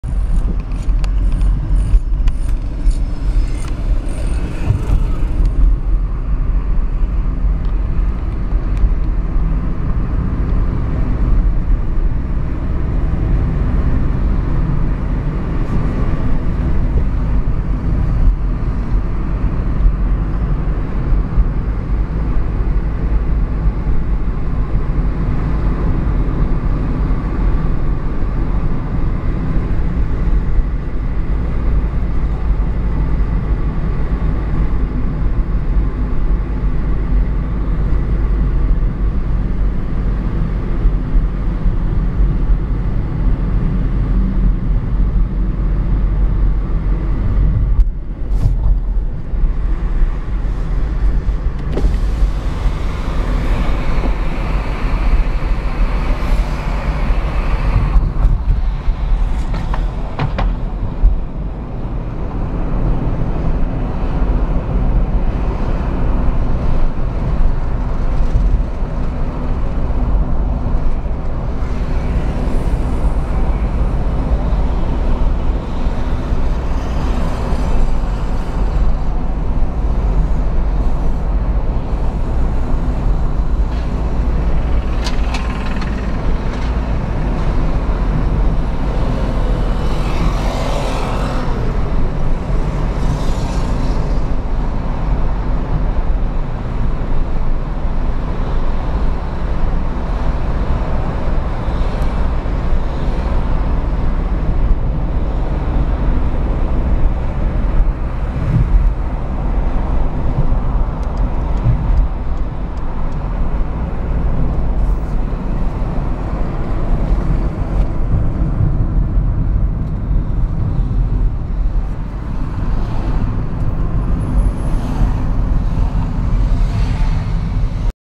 Nuselák 29.7.2011
průjezd po nových dilatacích a samozřejmě dále po tichém asfaltu...